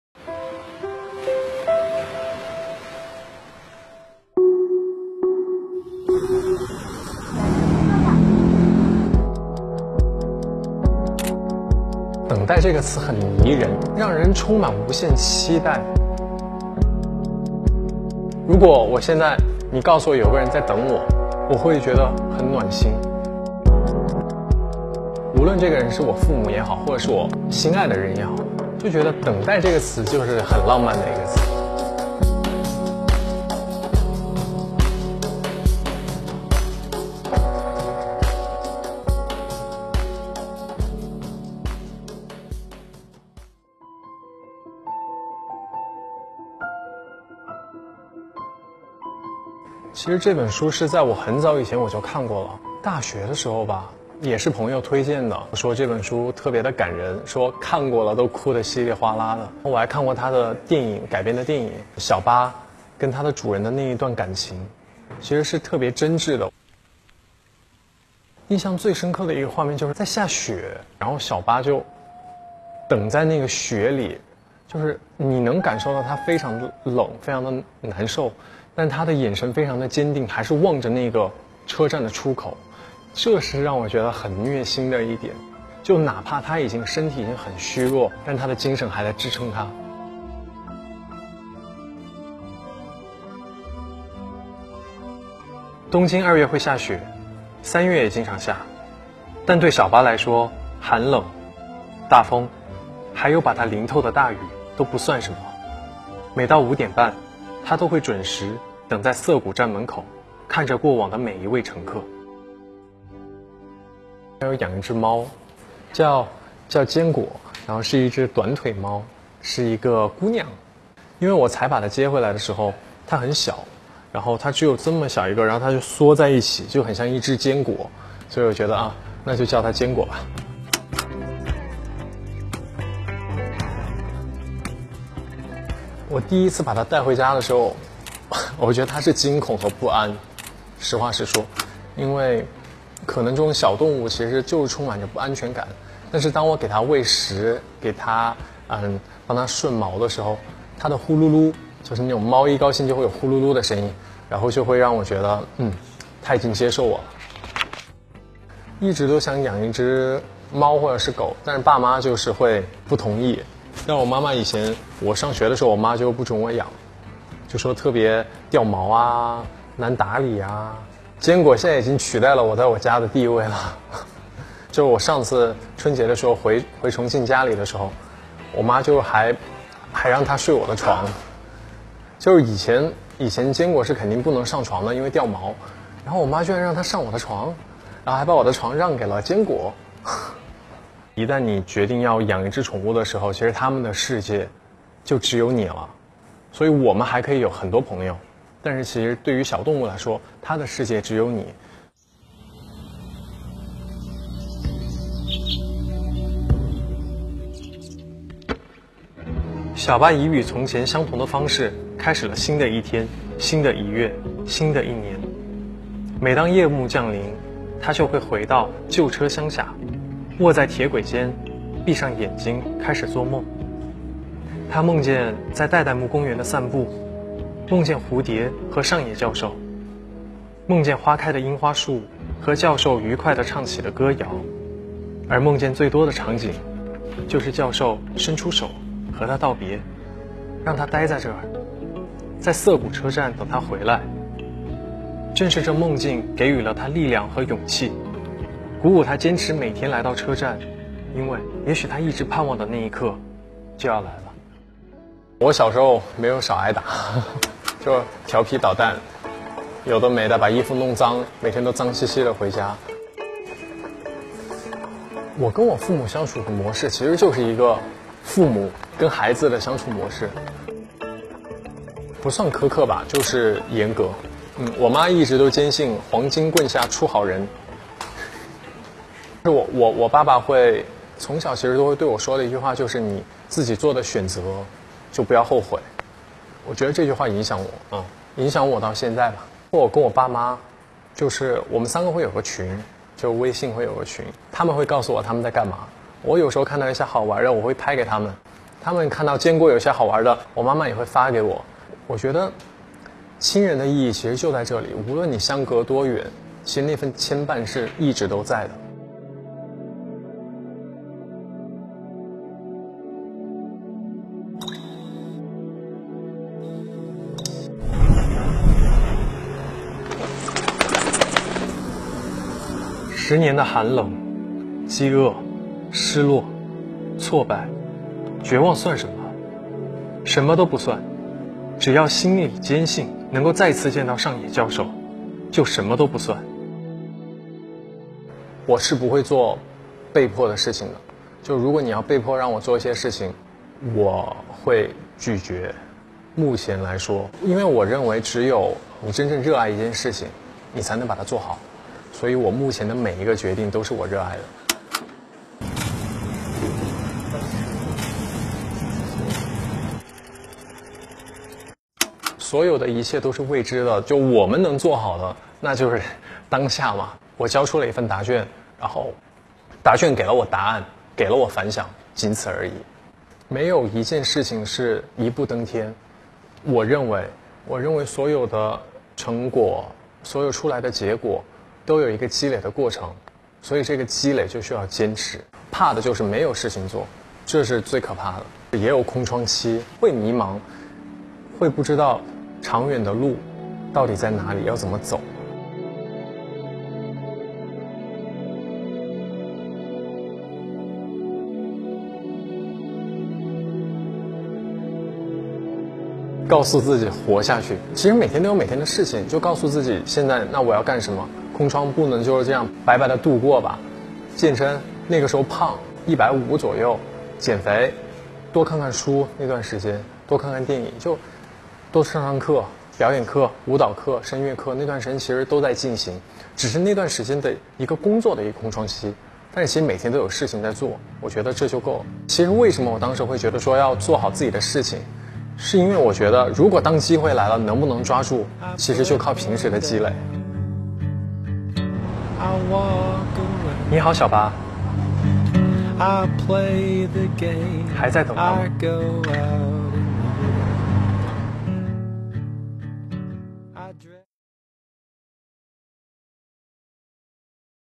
十点视频 weibo [2019.08.12] Xiao Zhan 肖战 read "Hachiko"
十点视频 weibo :
Waiting is a very beautiful word, such as the eight loyal dogs waiting for the owner, and the young man waiting for hard work to bear fruit. Time has never failed anyone who has given sincere heart. Tonight we walked into this warm boy together and read "The Hachiko" with him to listen to stories about waiting.